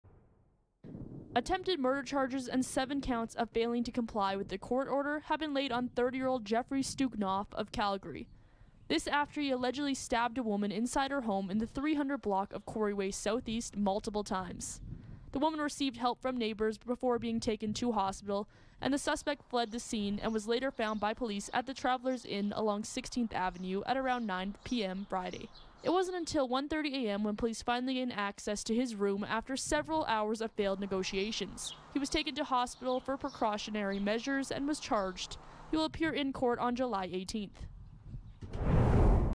Attempted murder charges laid for stabbing victim in SE Calgary
Attempted murder charges and seven counts of failing to comply with a court order have been laid on 30-year-old Geoffrey Stoochnoff.